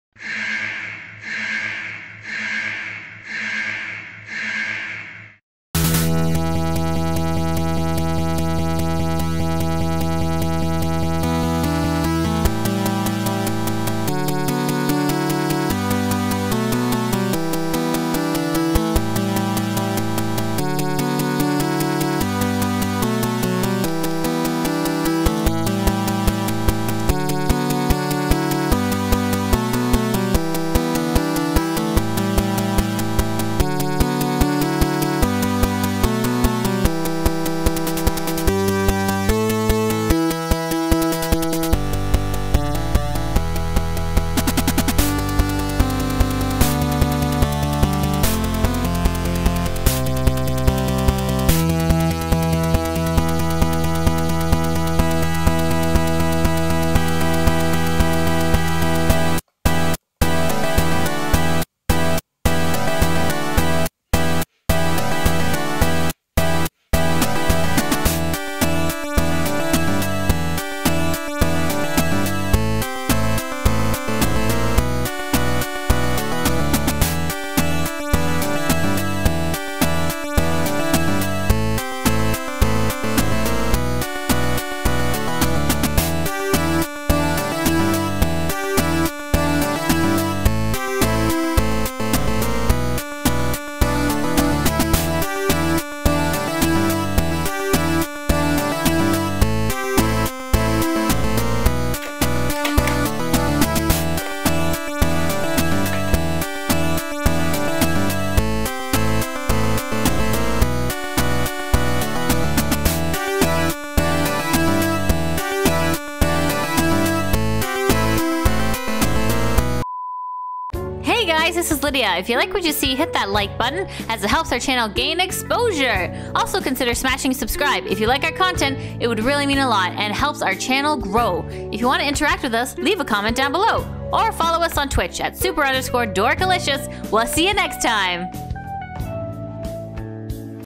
Interactive Smellovision, THE INTELLIVISION AMICO CAN BE YOUR FRIEND! ASSEMBLY & DEMO VIDEO!
In this video lets checkout the super secret console in development by yours truly, THE SMELLOVISION!!

NO IT DOESN'T STINK!

This console has taken us 2.5 years to develop and many hours of dedication but we hope that you like what we have to present for you today and that you will support us on kickstarter as we need your help to make this amazing console a reality.

So far I ate, assembled the console and played it. I feel the burden is now off me and onto you, my faithful followers to pay for my dream project.

Make sure you submit your applications for me to be a guest on your podcast today!

Gamer Tags:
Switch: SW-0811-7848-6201
PS4: Dorkalicious_Lyd